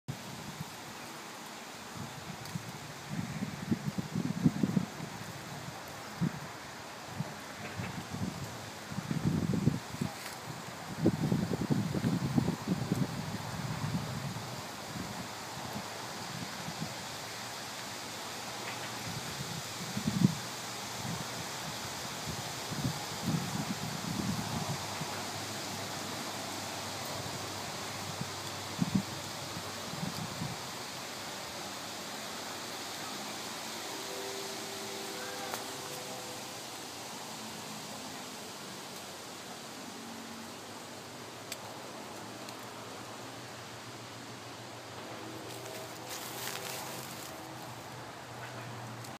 Definition of Fall - Montana
This is classic autumn, fall in action, on the eve before an enormous storm is to hit Montana. The trees are desperately trying to shed their leaves, as not to lose their branches under the weight of the snow. It's amazing how nature knows.